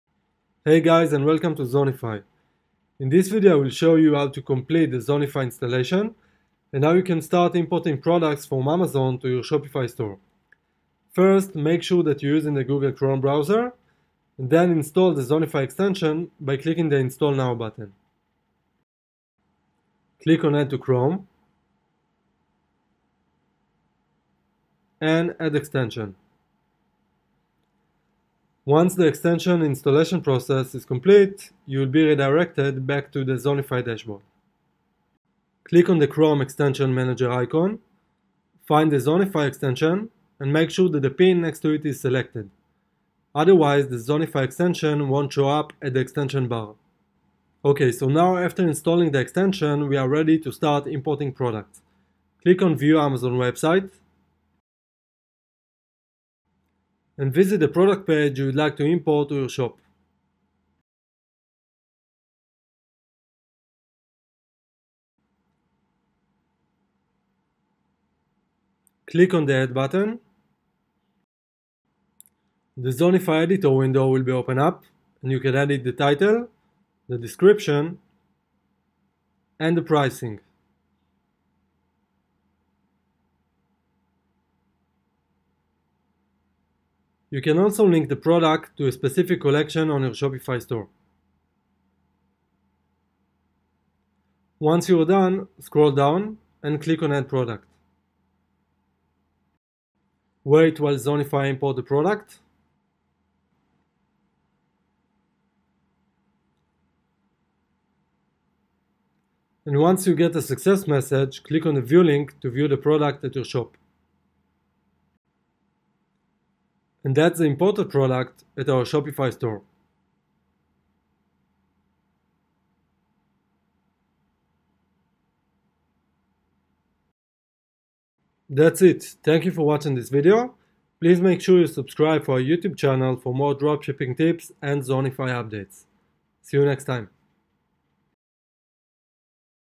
How to import products from Amazon to your Shopify dropshipping store?
Zonify allows you the ability to import products from Amazon with a click.

• Built-in editor for product descriptions. Edit title price images, tags category variants, and more.

• 1-Click to import products from the most popular Amazon suppliers.

More Questions?